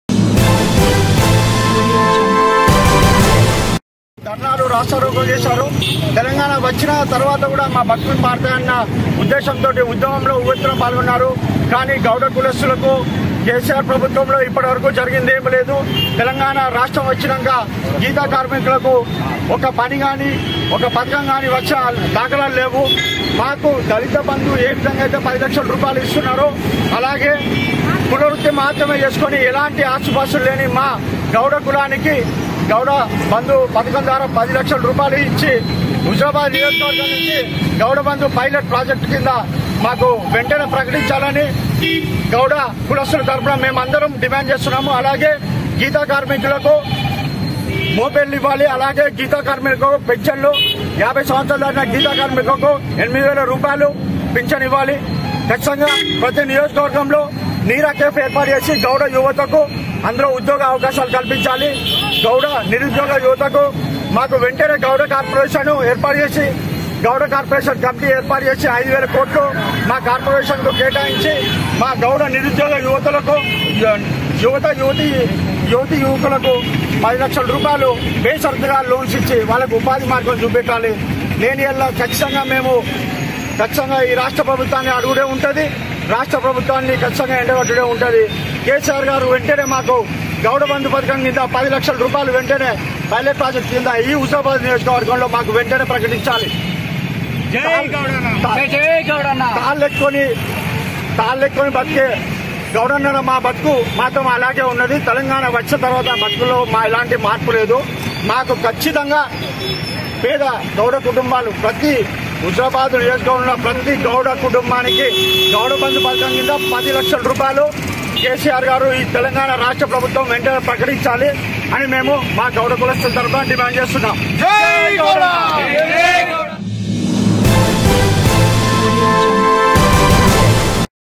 Gouds demanding CM KCR for Gouda Bandhu || Castle News9
News # Gouds demanding CM KCR for Gouda Bandhu